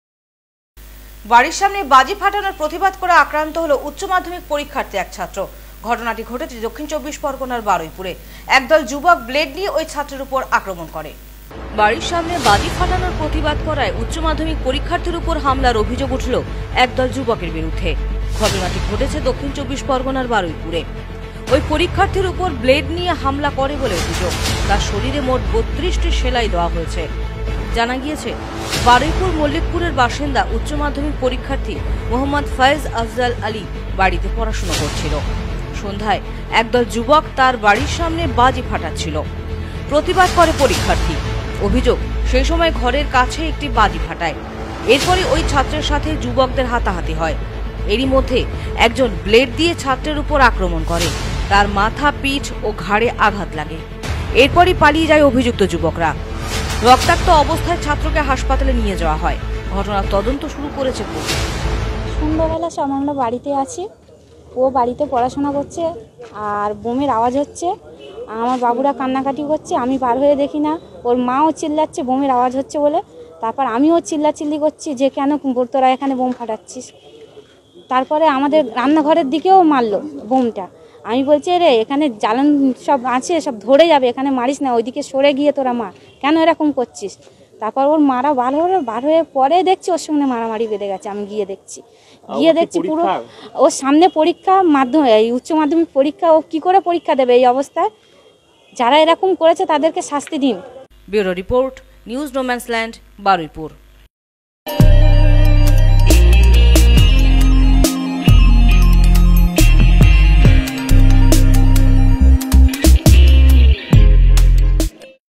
বাজি ফাটানোর প্রতিবাদ করে আক্রান্ত হল উচ্চ মাধ্যমিক পরীক্ষার্থী ।
বাড়ির সামনে বাজি ফাটানোর প্রতিবাদ করে আক্রান্ত হল উচ্চ মাধ্যমিক পরীক্ষার্থী এক ছাত্র। ঘটনাটি ঘটেছে দক্ষিণ ২৪ পরগনার বারুইপুরে। ব্লেড নিয়ে ছাত্রের উপর আক্রমণ করে একদল যুবক।